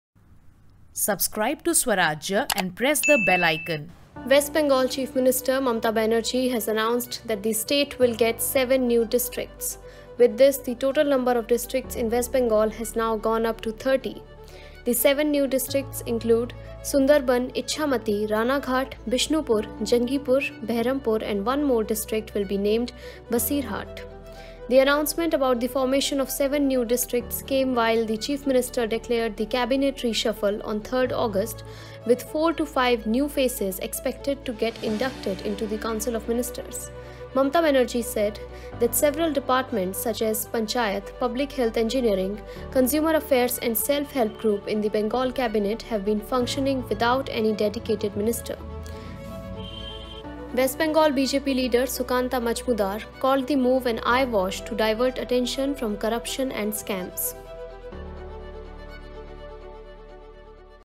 West Bengal To Get 7 New Districts, Total Number Rises To 30
West Bengal Chief Minister Mamata Banerjee has announced that the state would get seven new districts. With this, the total number of districts in the state has now gone up to 30. The six of the seven new districts that were announced are - Sunderban, Ichhamati, Ranaghat, Bishnupur, Jangipur, and Behrampur. One district will be named Basirhat.

The announcement about the formation of seven new districts came while the chief minister declared the cabinet reshuffle. She added that the new ministers – four to five of them – would take oath on 3rd August.

Mamata Banerjee also said that several departments such as Panchayat, Public Health Engineering, Consumer Affairs & Self Help Group in the Bengal cabinet have been functioning without any dedicated minister.

West Bengal BJP leader Sukanta Majumdar called the move an 'eyewash' to 'divert attention' from corruption and scams.